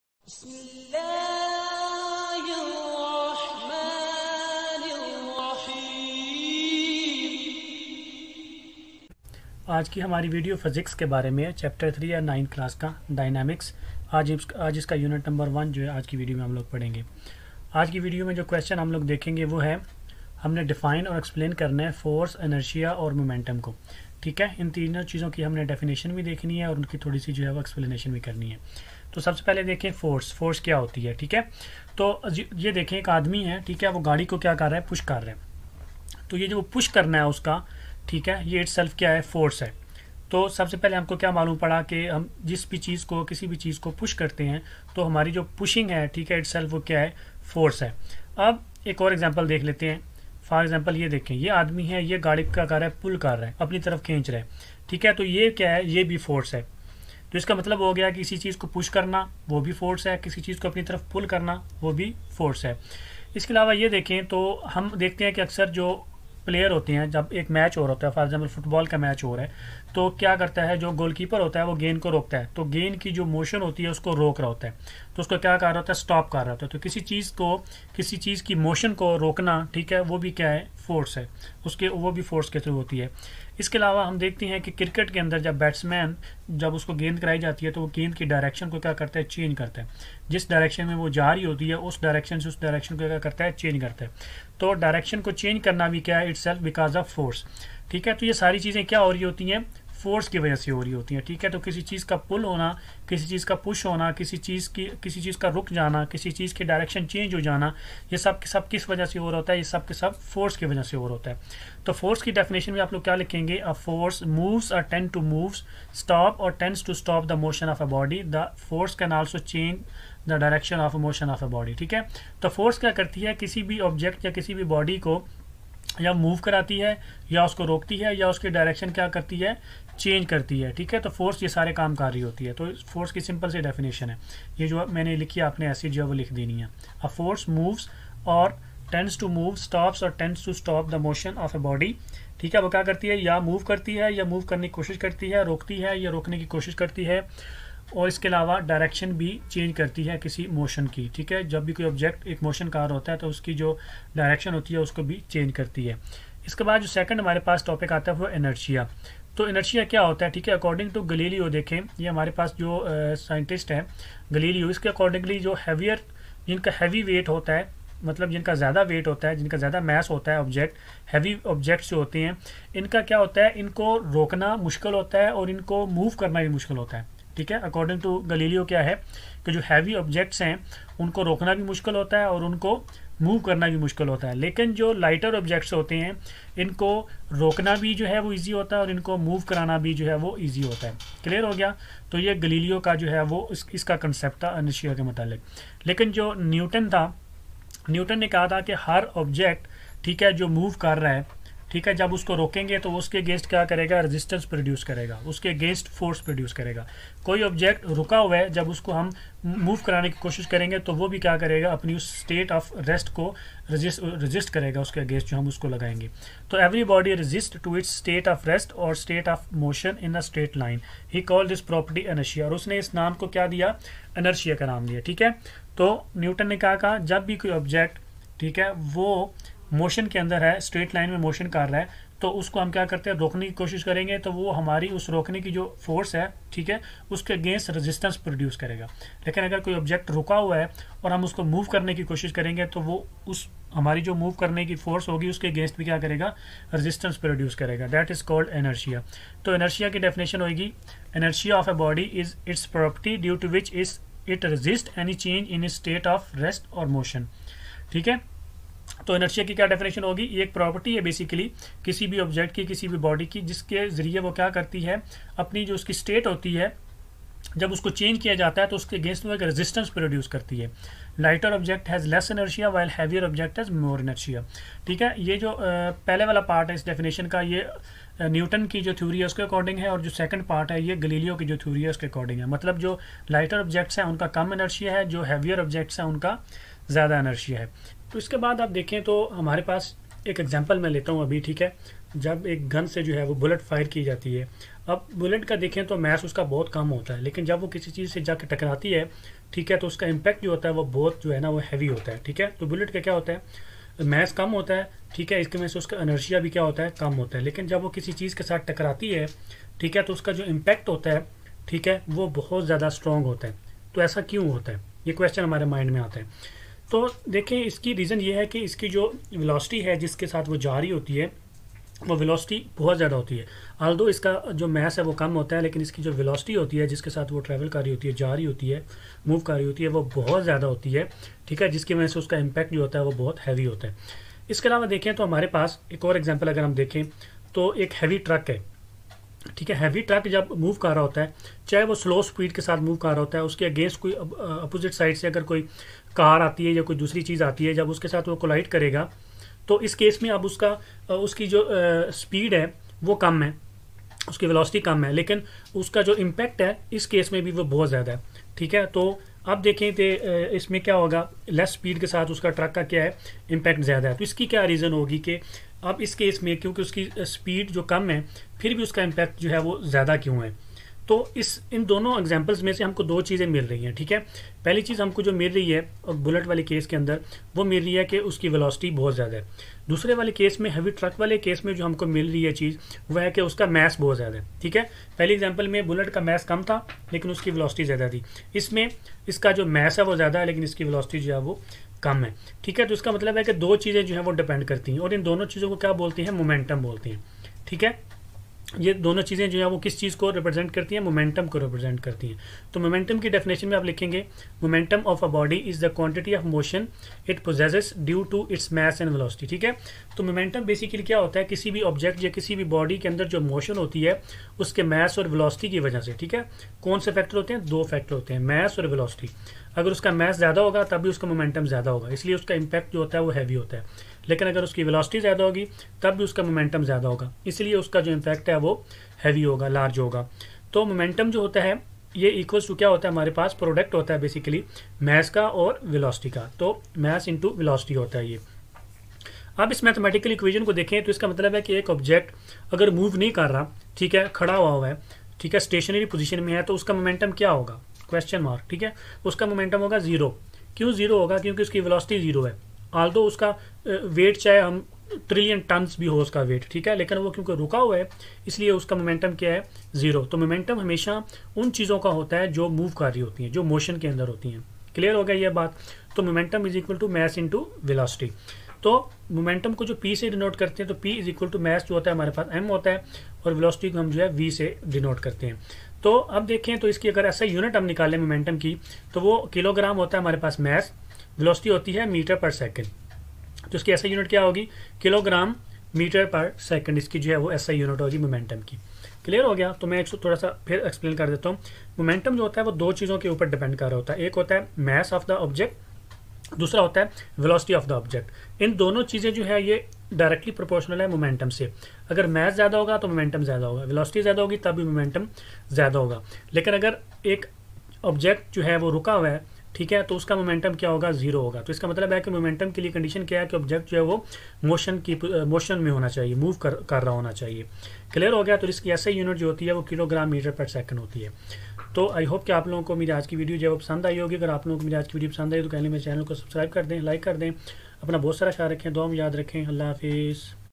Force, Inertia & Momentum - Unit 3 (Lecture-1) ||Dynamics||Physics-9th Class|Toqeer Academy
Definition and explanation of force
what is inertia?
Theory of Galileo about Inertia
concept of Newton about Inertia
and
what is momentum?
Why does impact of bullet is so strong while its mass is very small?